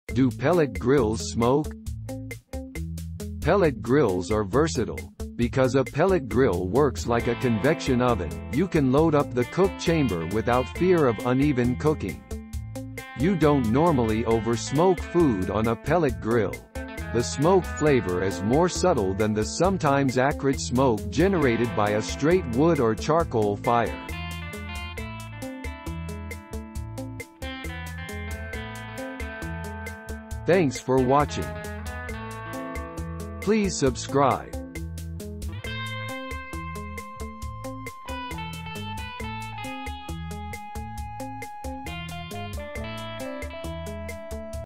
Do pellet grills smoke?
A Guide to Pellet Grills - Barbecuebible.com

Pellet grills are versatile. ... Because a pellet grill works like a convection oven, you can load up the cook chamber without fear of uneven cooking. You don't normally over-smoke food on a pellet grill. The smoke flavor is more subtle than the sometimes acrid smoke generated by a straight wood or charcoal fire.